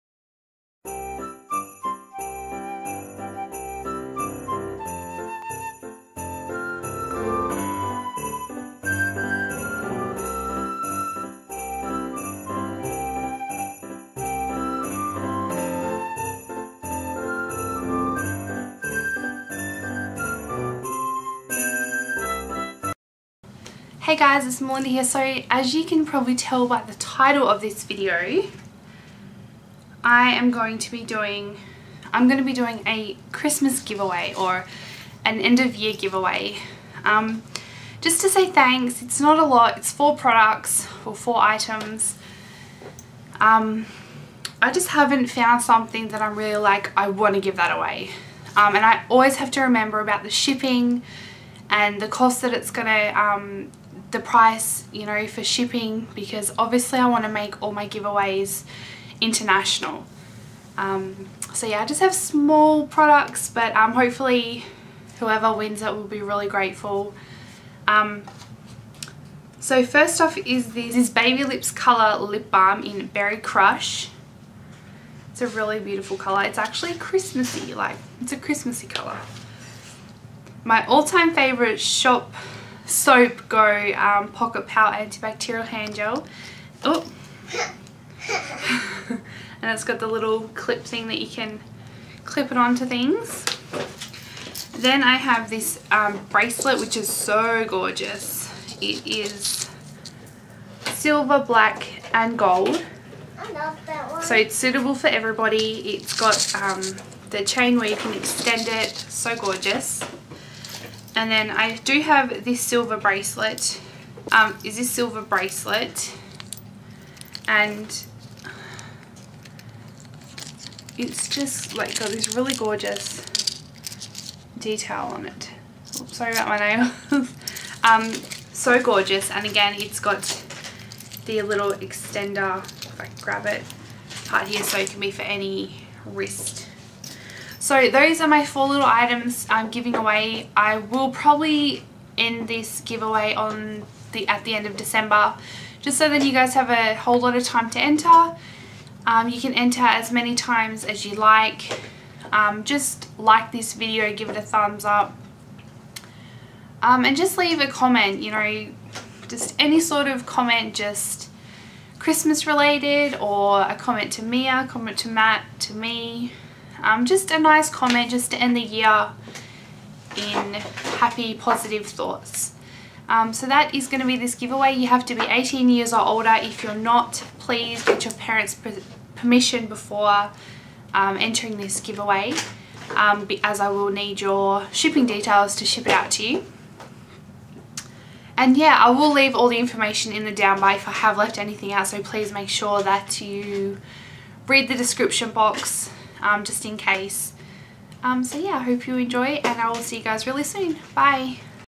VLOGMAS GIVEAWAY ****OPEN****
Thankyou for watching my give away video i just want to thank everyone for all your support for the past year =)
TO ENTER-
AND LEAVE A NICE COMMENT SO WE CAN END THIS YEAR WITH HAPPY POSITIVE THOUGHTS.
IF YOU ARE UNDER 18 PLEASE ASK YOUR PARENTS PERMISSION BEFORE ENTERING.
YOU CAN COMMENT AS MANY TIMES AS YOU LIKE BUT PLEASE TRY NOT TO SPAM.

THIS GIVEAWAY WILL END ON THE 31ST DECEMBER MIDNIGHT WESTERN AUSTRALIA STANDARD TIME

THANKYOU ALL ONCE AGAIN xo

Let me know if you have any questions or video requests =)

EVERYTHING WAS PAID FOR BY ME UNLESS STATED OTHERWISE

XO Mel

Pintrest- Missmellyful

Instergram- Mummymelly